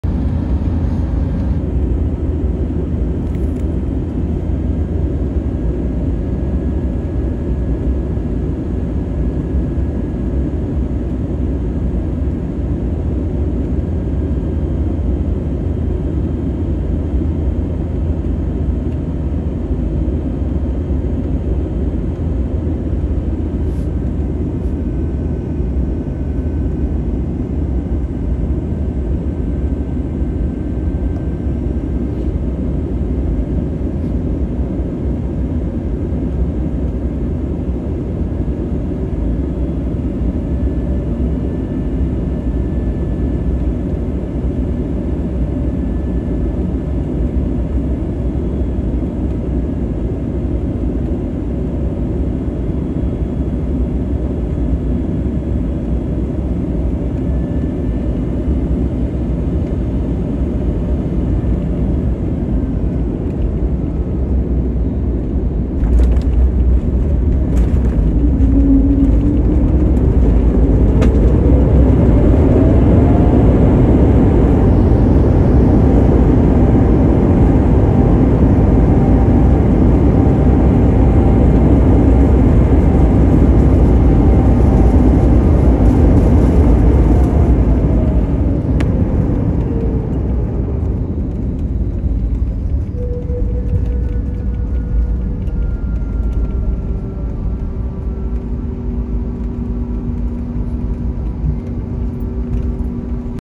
Da landing in Manila video bc why not
Btw this vid is where I arrived to Manila at 12 AM in Monday Aug 26th ig